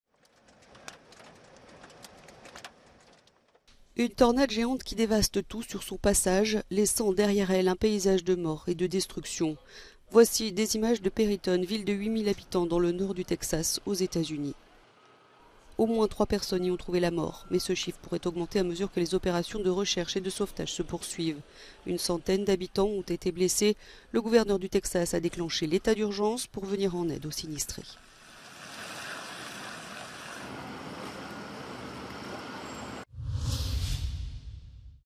Tornade aux Etats-Unis : la petite ville de Perryton est ravagée, il y a au moins 3 morts
lLa petite ville de Perryton, au Texas, a été ravagée. Il y a au moins 3 morts et une centaine de blessés.